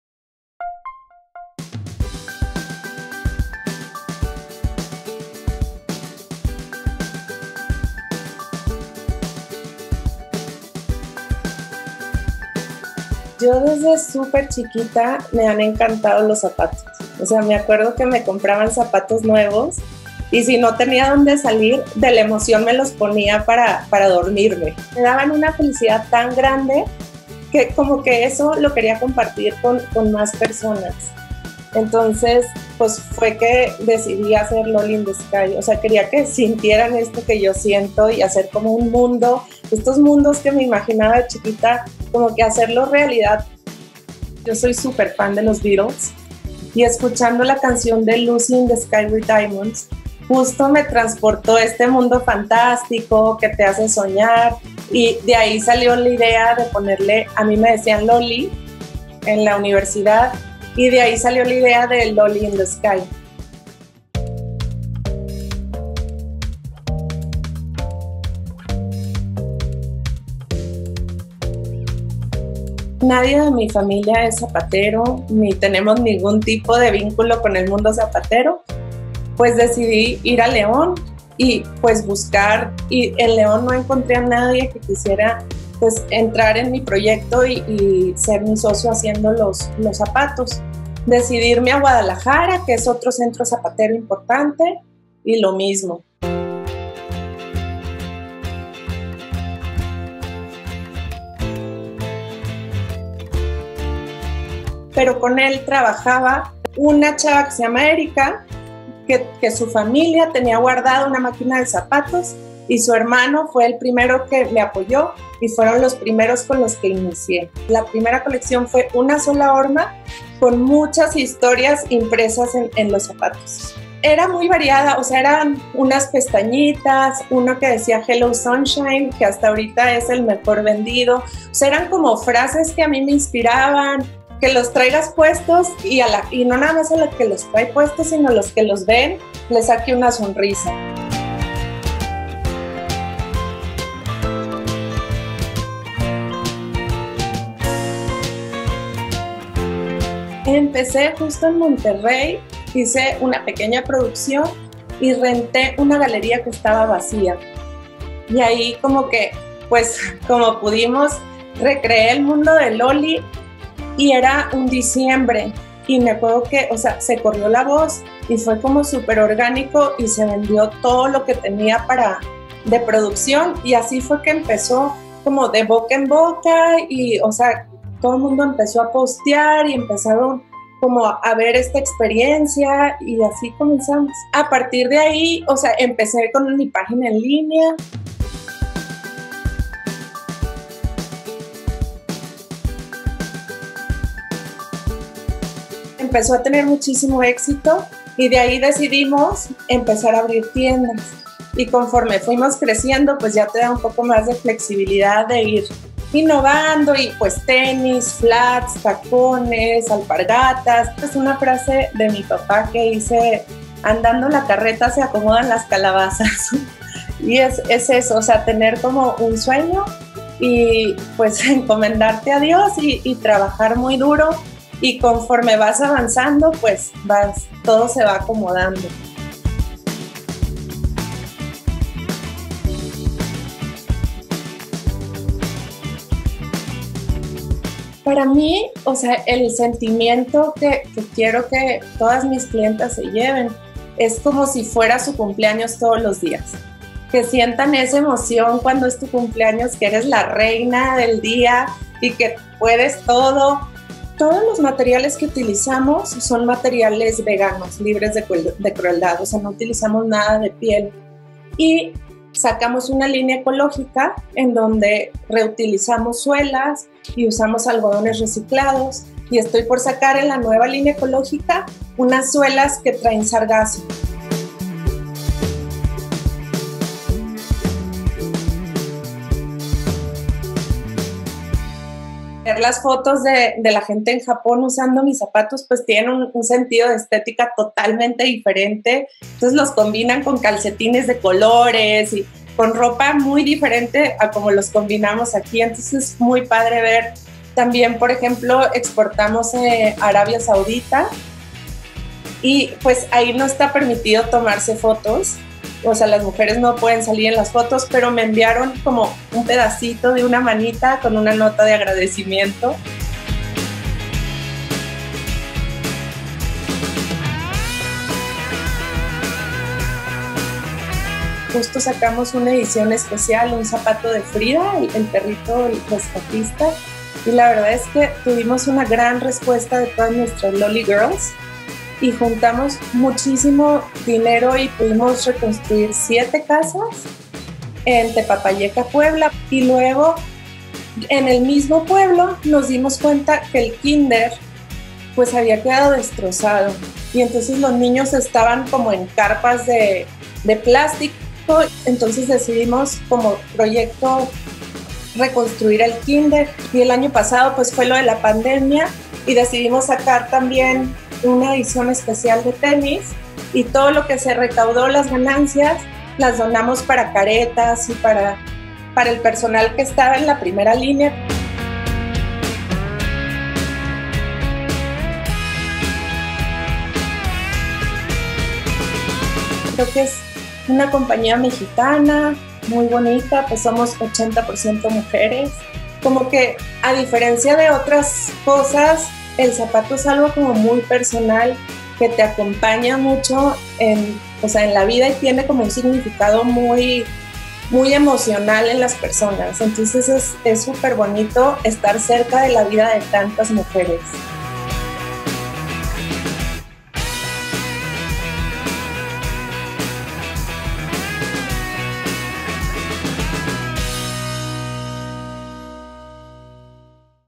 Lorena Vázquez: El paso fantástico de Loly | Perfiles e Historias
En el 2013 la regia Lorena Vázquez lanzó Loly in the sky en Monterrey. Hoy, sus zapatos, accesorios y fragancias, se conocen alrededor del mundo.